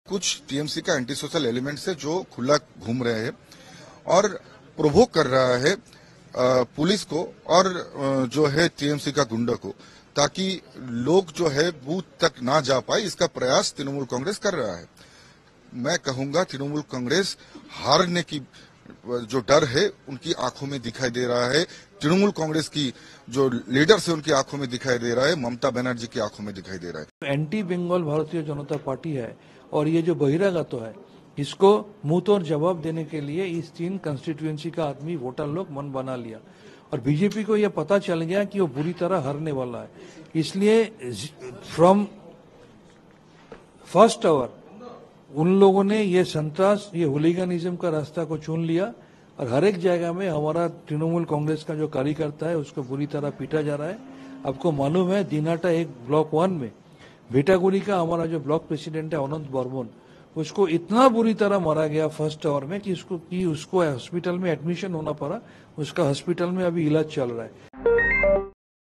BJP vs TMC: बंगाल हिंसा पर बीजेपी-टीएमसी में चले जुबानी तीर, लगाए ये आरोप.
बीजेपी नेता निसिथ प्रामाणिक के आरोपों पर टीएमसी नेता शांतनू सेन ने पलटवार किया. दरअसल, निसिथ प्रामाणिक ने बंगाल में हिंसा के लिए टीएमसी को जिम्मेदारी बताया जिस पर टीएमसी नेता शांतनू सेन ने पलटवार किया.